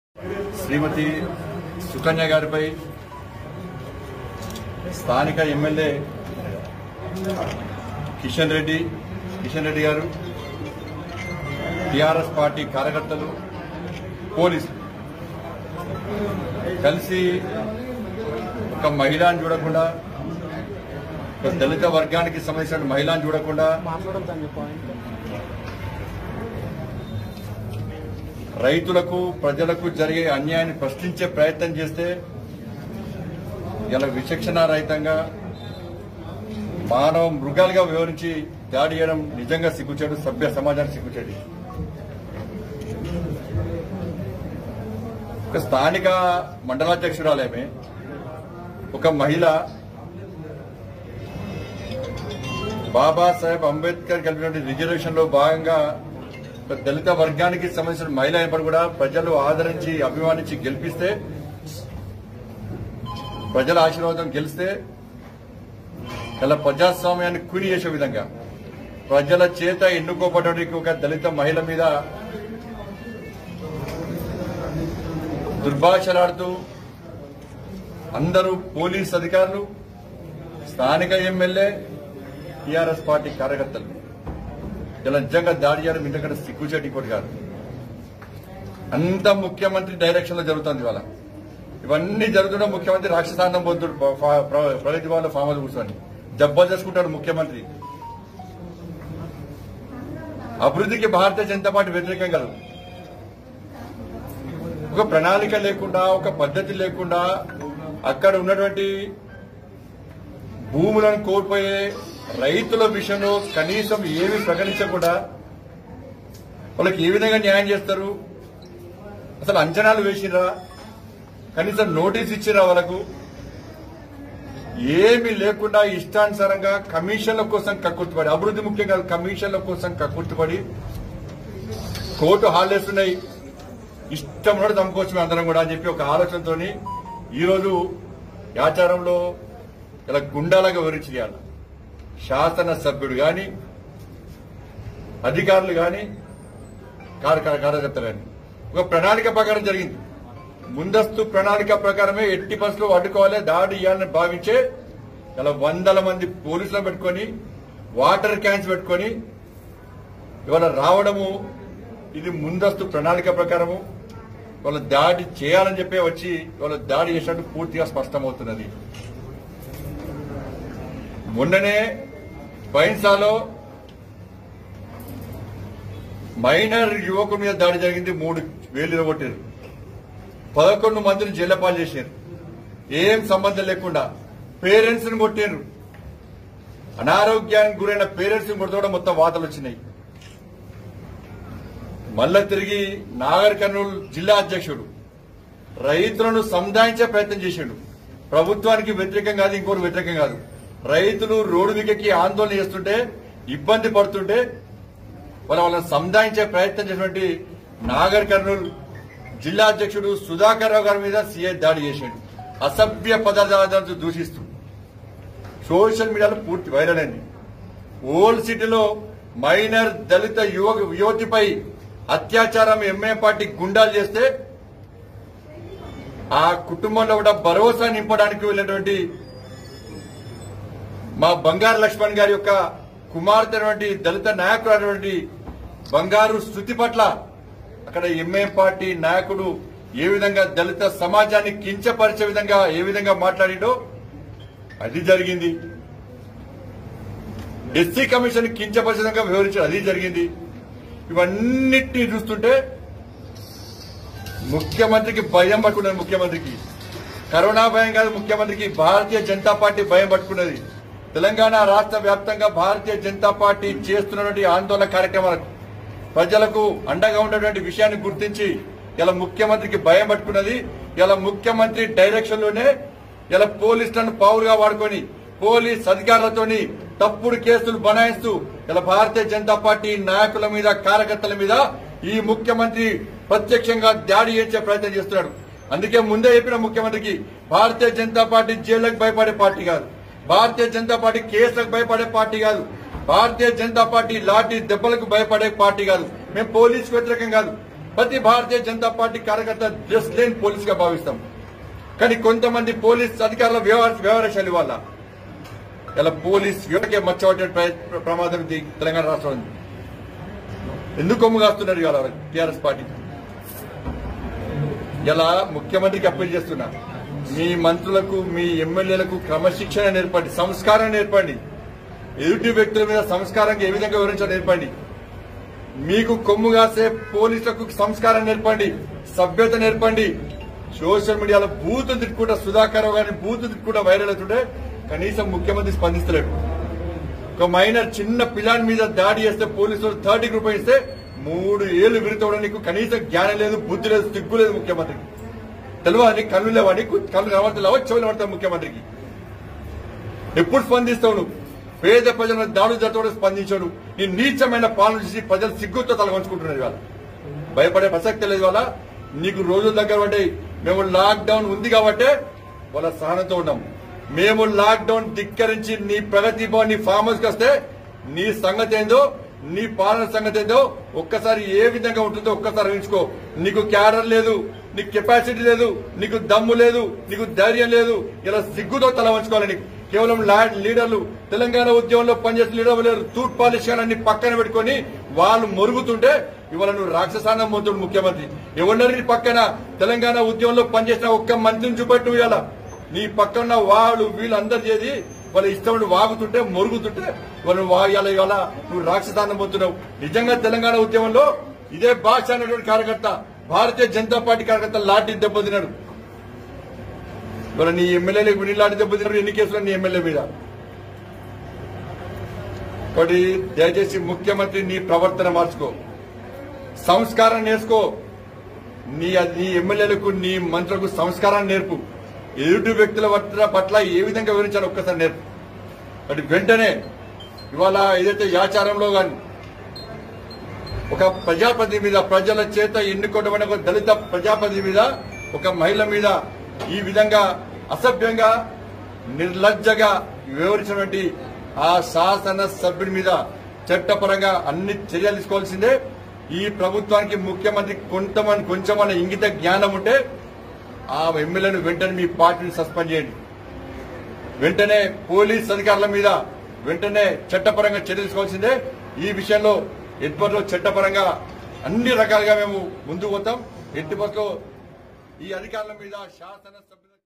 Bandi Sanjay Says KCR Affraid of BJP Party Rise In Telangana |MPP Sukanya|TRS MLA Manchi KishanReddy
Watch Bandi Sanjay Says KCR Affraid of BJP Party Rise In Telangana and serious warning to cm KCR